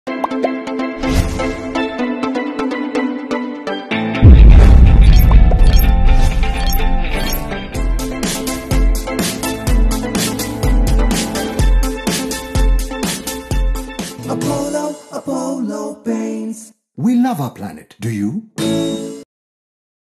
Apollo paints - Sparkle
Sparkle PU-Acrylic Floor Coat Heavy Duty is a special paint with new innovative PU Acrylic technology to impart aesthetic look to your floor surfaces.